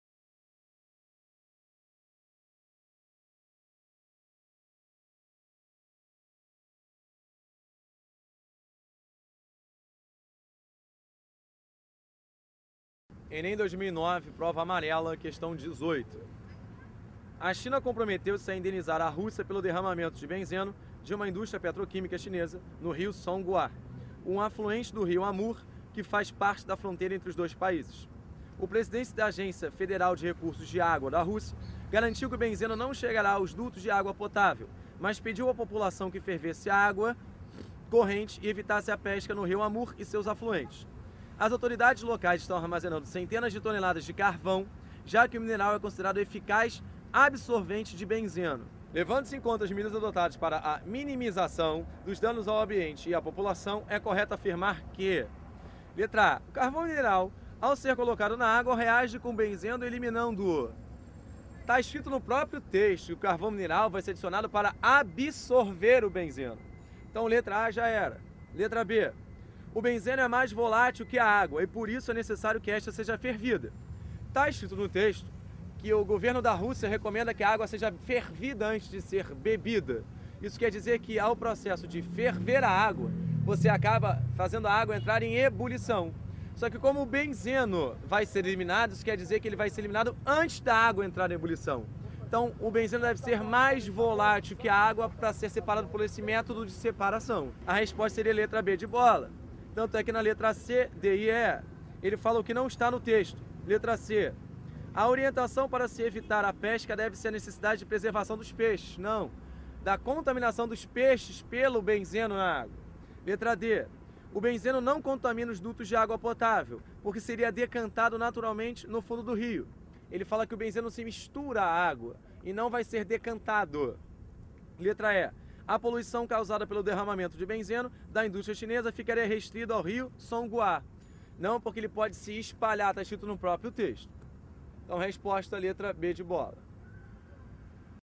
ENEM 2008 - QUÍMICA - Prova Amarela: Questão 18
Nessa videoaula de QUÍMICA, o prof. Carlos Vitorino resolve para você a Questão 18 da Prova Amarela do ENEM 2008 sobre cinética química.